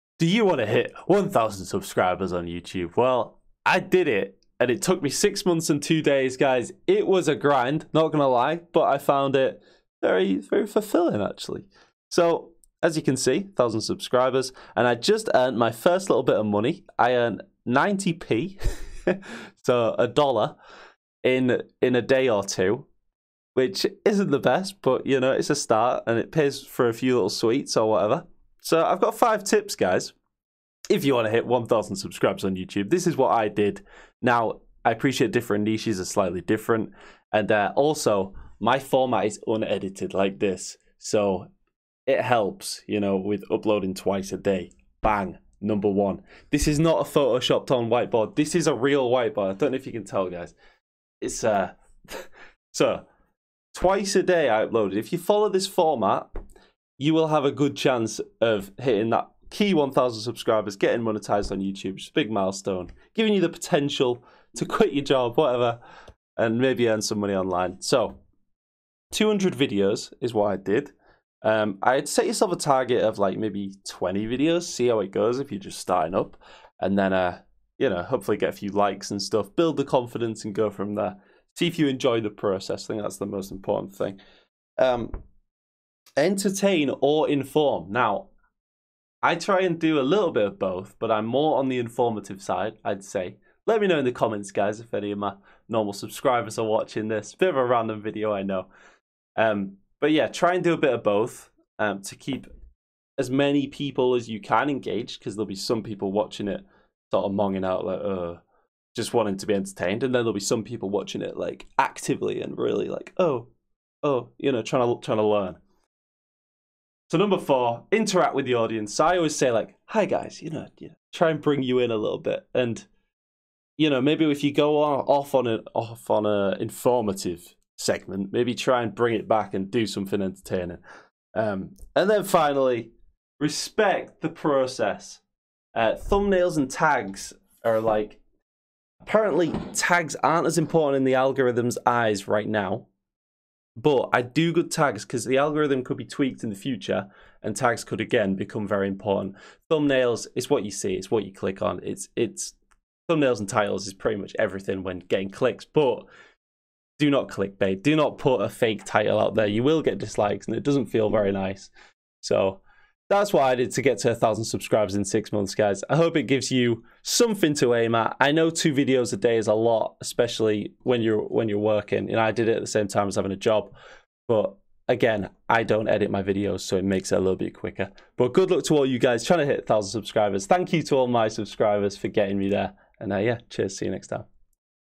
How I Hit 1000 Subscribers in 6 Months! (Playing Chess!?)
Welcome to CHESS GOALS, in this video I talk about how I hit 1000 Subscribers in 6 months playing chess

To participate in the weekly Sub Battles- JOIN THE FREE DISCORD